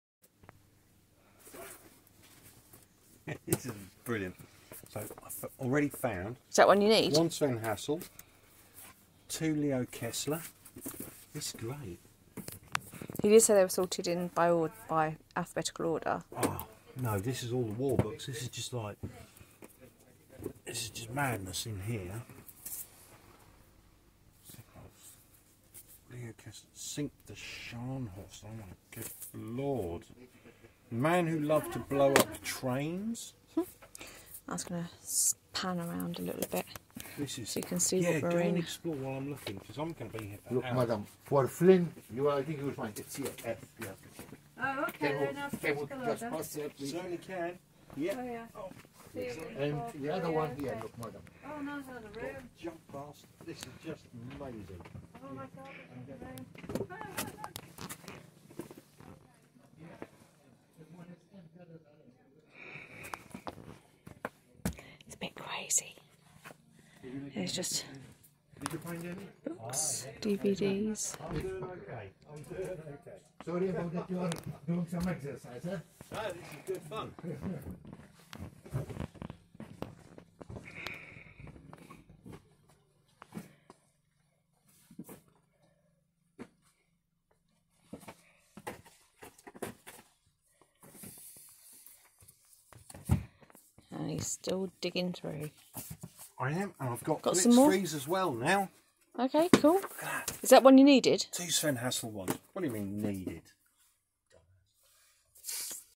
Finding Sven Hassel & Leo Kessler novels in the best secondhand book shop in the world Bugibba Malta
Finding Sven Hassel & Leo Kessler novels in the best book shop in the world Bugibba Malta